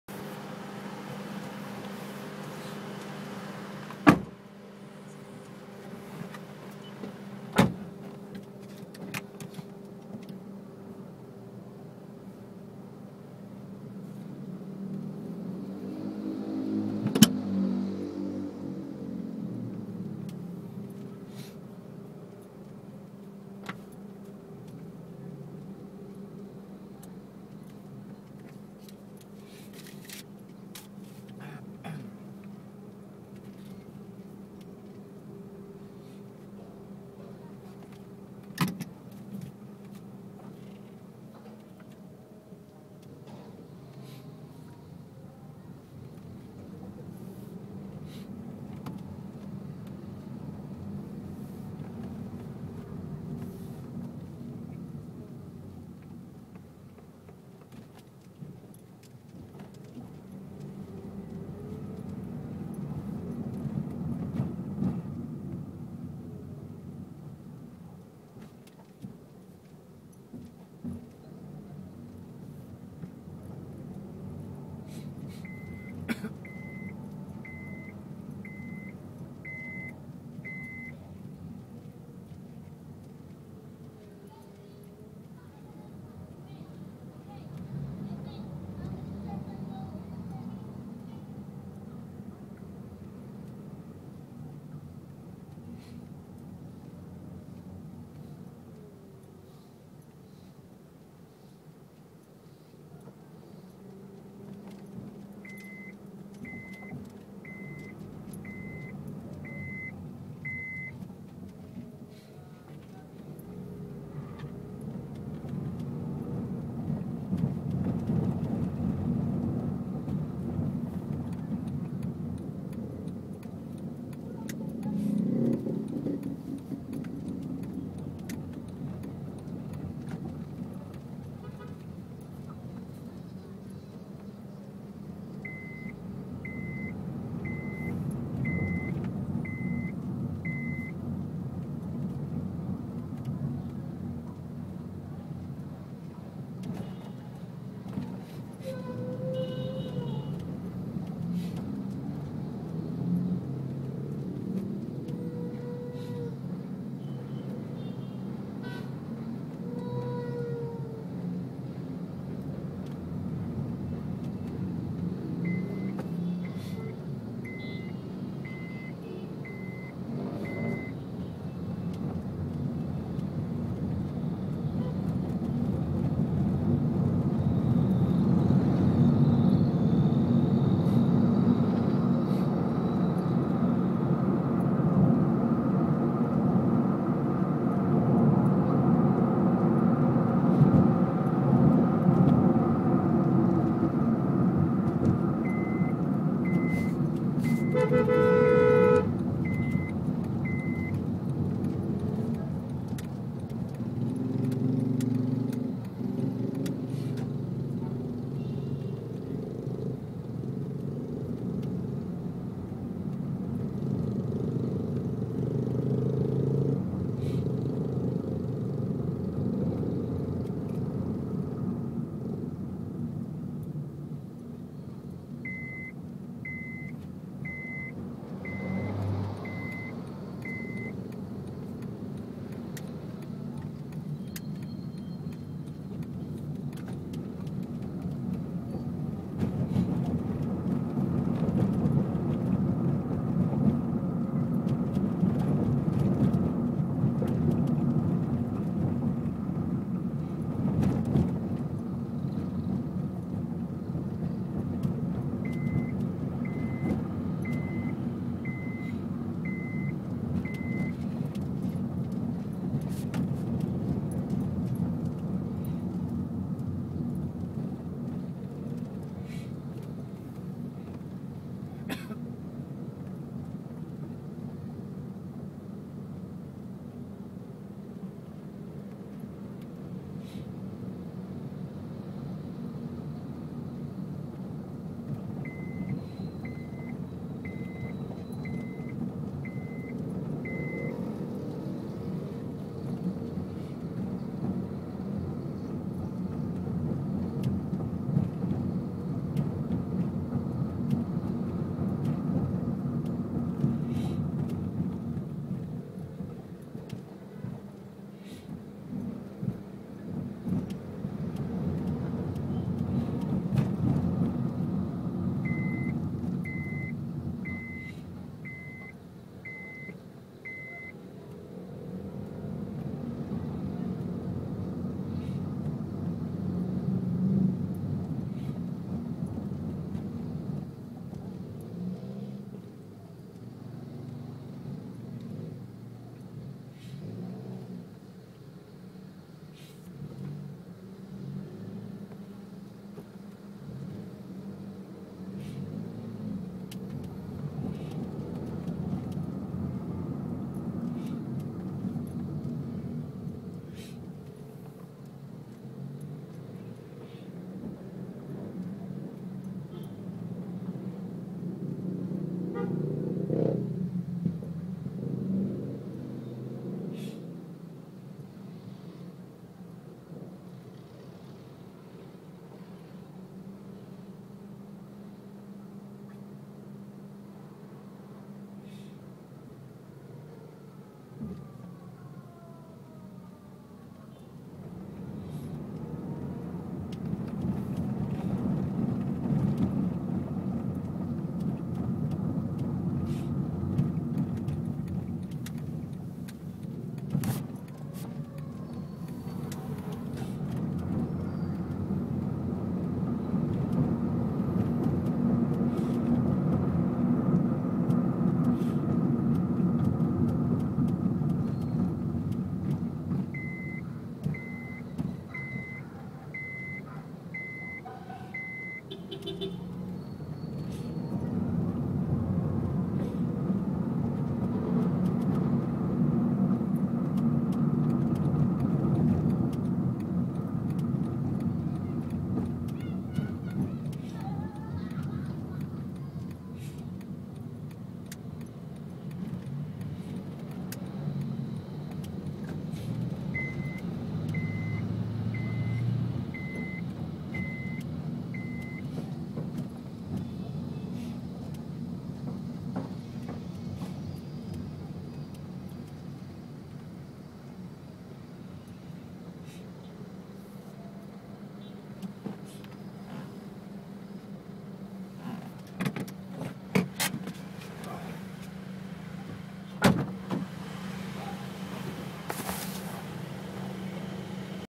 2006 Honda Civic - Manila Driving (POV)
Driving from home to get my eyeglasses on a crowded street of Metro Manila